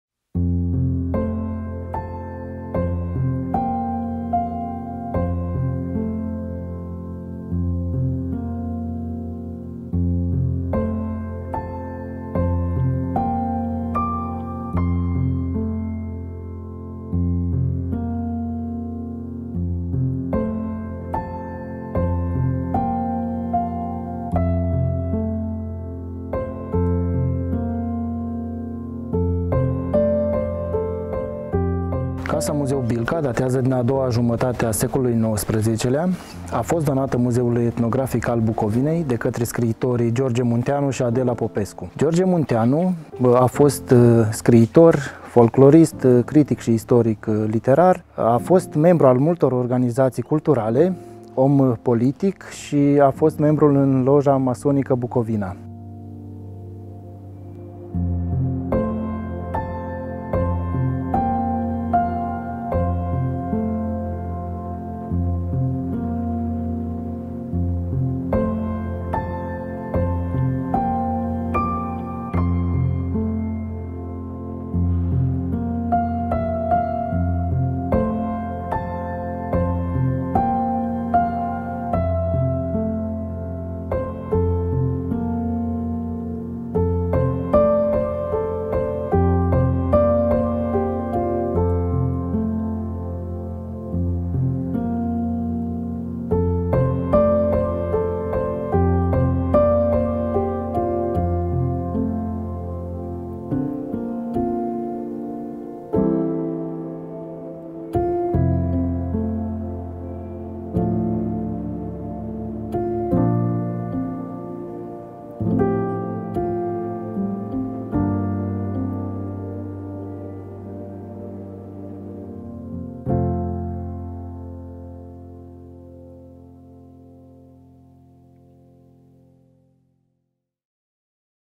Casa-muzeu „George Muntean” Bilca din județul Suceava | Muzee de la sat - Moldova
În anul 2000, scriitorii George Muntean și Adela Popescu donează această gospodărie către Muzeul Național al Bucovinei. Casa de locuit și anexele gospodărești (grajdul, poarta, gardul din scândură acoperit cu draniță, fântâna cu cumpănă) reprezintă arhitectura specifică și organizarea arealului de locuit specific zonei etnografice Rădăuți, Bucovina. Interiorul este organizat cu mobilier țărănesc, textile de interior, icoane, obiecte de uz gospodăresc parte din ele aparținând familiei, iar altele au fost colecționate de George Muntean.

Explorează turul virtual 3D interactiv pe platforma Muzee de la sat

[EN] In 2000, the writers George Muntean and Adela Popescu donated this household to the Bucovina National Museum. The house and the household annexes (the stable, the gate, the wooden fence and the balance-well) represent the particular architecture and the organisation of the living space specific to the ethnographic area of Rădăuți, Bucovina. The interior is arranged with rural furniture, fabrics, icons, household tools. Some of these things belonged to the family and some were collected by the writer.